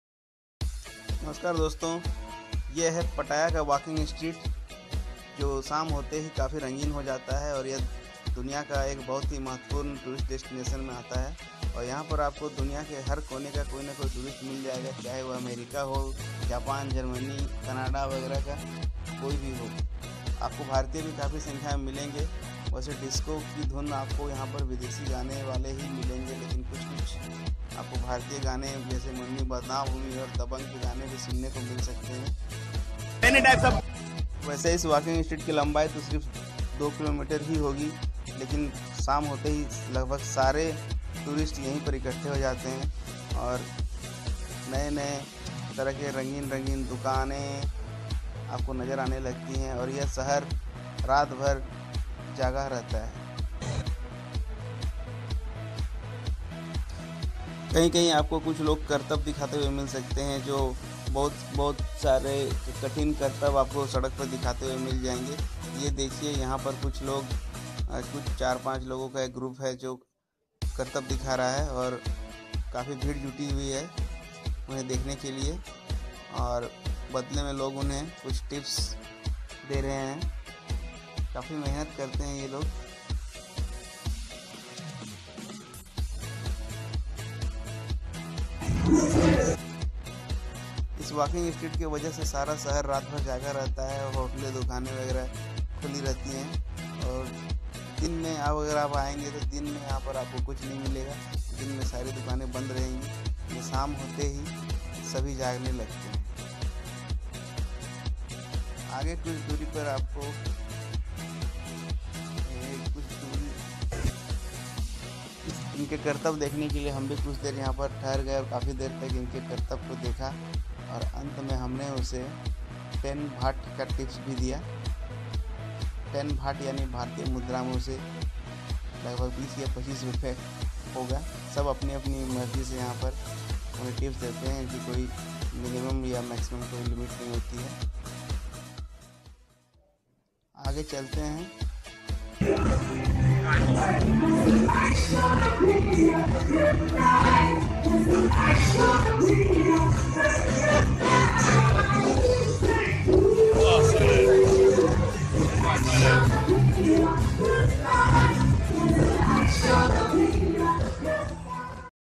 Pattaya Walking Street Thailand
This is Walking Street in Pattaya, Thailand. It is one of the most famous tourist destinations of the world.
In this channel, I will take you to various tourist places of India and share my personal travel experiences.